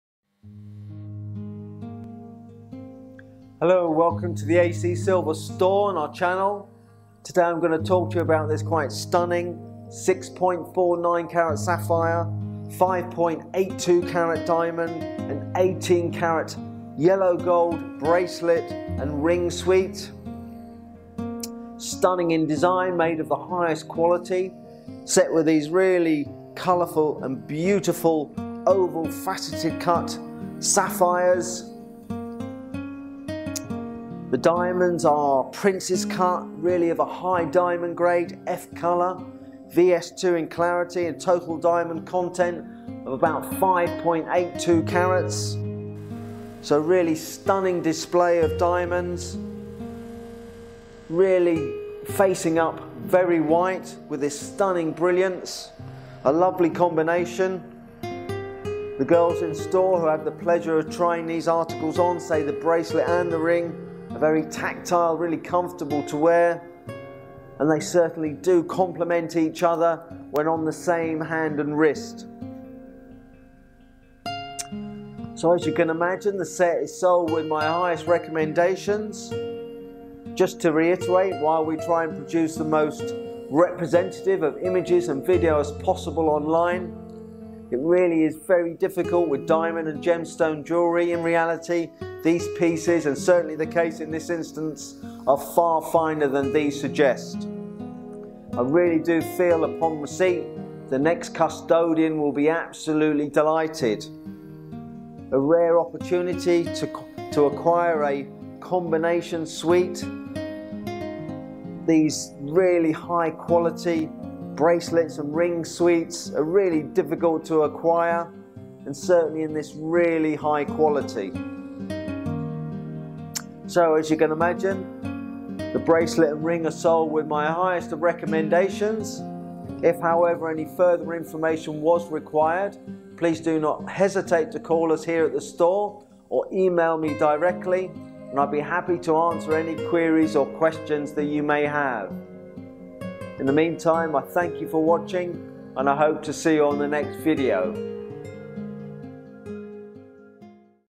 6.49 CT SAPPHIRE, 5.82 CT DIAMOND AND 18 CT YELLOW GOLD JEWELLERY SET - VINTAGE CIRCA 1990
A stunning, fine and impressive vintage 6.49 carat sapphire, 5.82 carat diamond and 18 carat yellow gold bracelet and ring jewellery suite; part of our vintage sapphire jewellery collections at AC Silver.

This stunning, fine and impressive vintage sapphire jewellery set consists of a ring and a bracelet; each crafted in 18ct yellow gold.

The articles of this vintage jewellery suite are embellished with tension set impressive oval faceted sapphires, totalling 6.49cts.

The feature gemstone to the ring is flanked with eight impressive channel set princess cut diamonds, in a linear formation.

The plain ring shoulders integrate with the main setting and blend seamlessly with the hallmarked shank.

The vintage bracelet is comprised of seven sapphires, alternating with linear arrays of six channel set princess cut diamonds, reflecting the same design as the ring.

This impressive vintage diamond and sapphire bracelet secures to the reverse with a discreet, integrated clasp and benefits from the added security of a hinged safety clasp.

The vintage ring and bracelet are each hallmarked with the maker's mark (© QUAD ®) and 18ct gold fineness mark (750).

This impressive jewellery set is supplied with an IDGL diamond and gemstone grading report card.

Images do not always reflect the true colour and brilliance of gemstones and diamonds. The video however provides a truer representation of the actual colour and showcases each stone.

Condition:
This vintage sapphire jewellery set is excellent quality, set with impressive sapphires displaying fine colour, brilliance and presence.

The impressive high colour graded diamonds display a fine white colour, brilliance and presence.

The bracelet and ring are an excellent gauge of gold and are in excellent condition, with clear hallmarks to the interior of the shank and reverse of the clasp.

SHOP Jewellery
🛒 acsilver.co.uk
SKU C8052